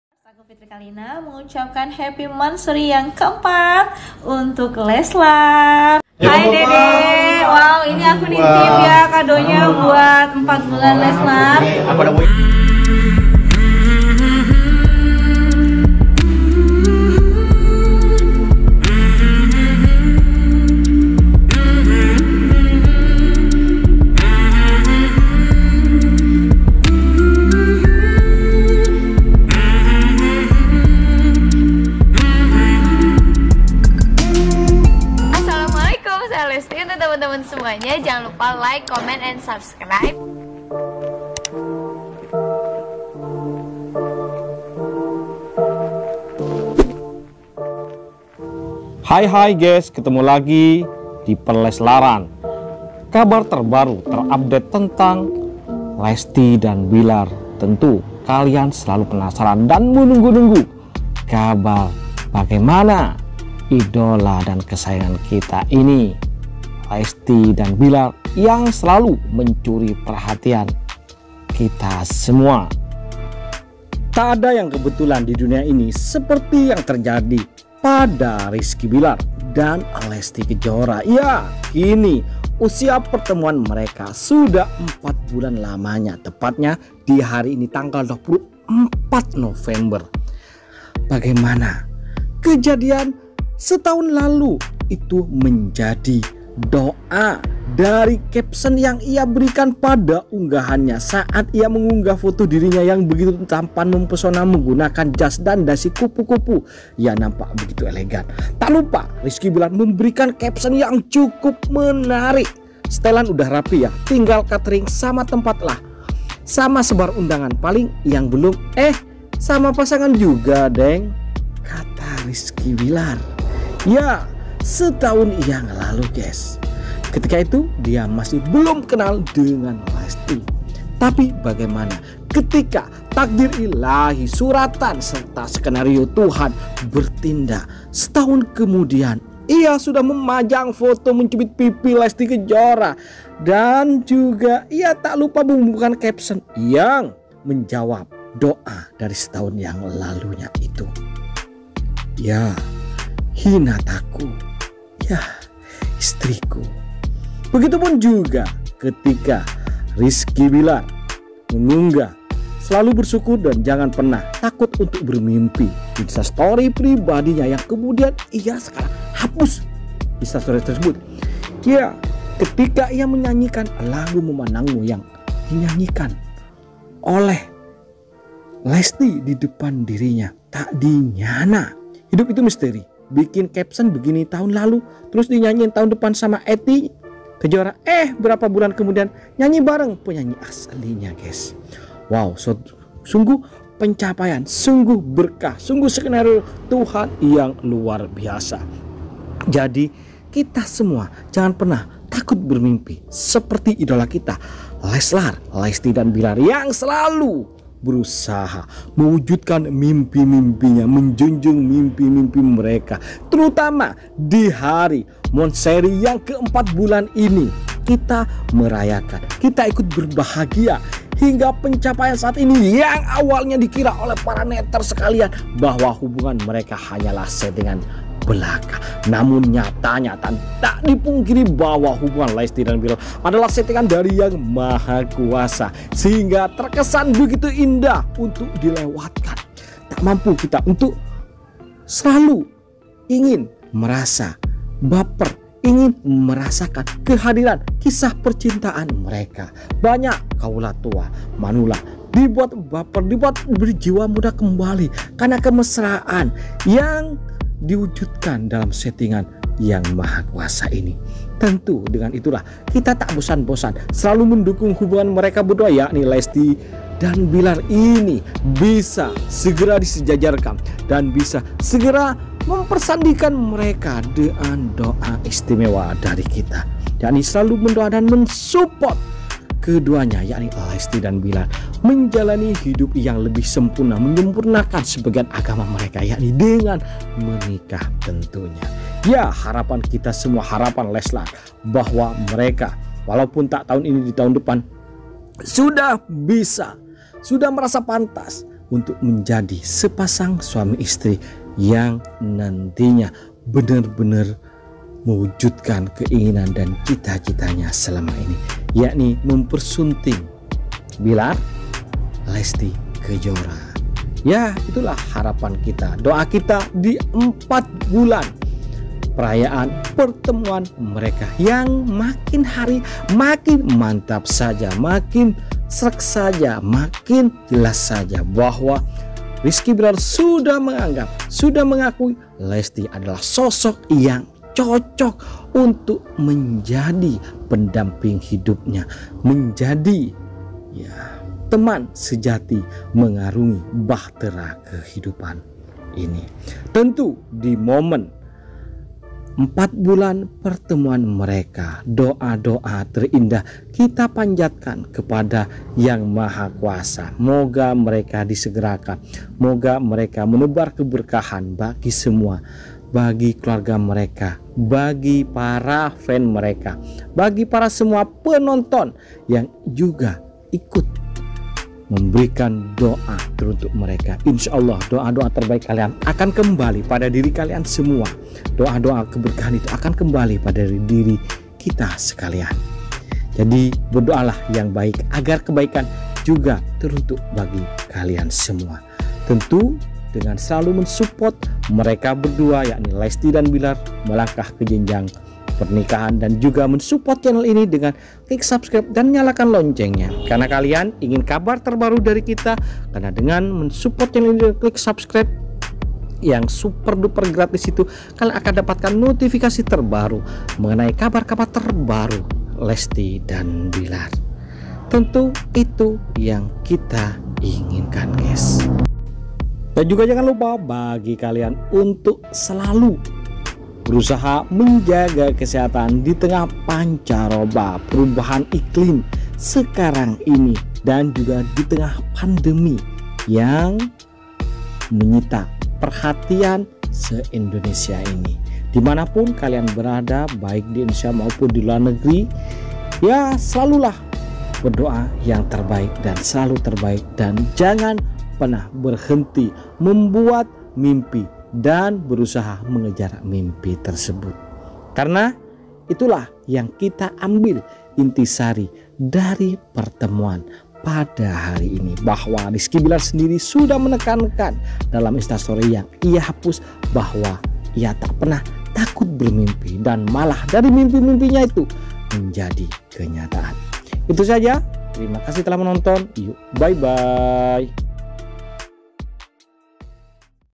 LESTi Dibuat HARU oleh Rizky Billar dg KEJUTAN ROMANTiS? Semua Diluar SKENARIO!! Tapi yg TERJADI??
Jangan Lupa: _
Thankz!!..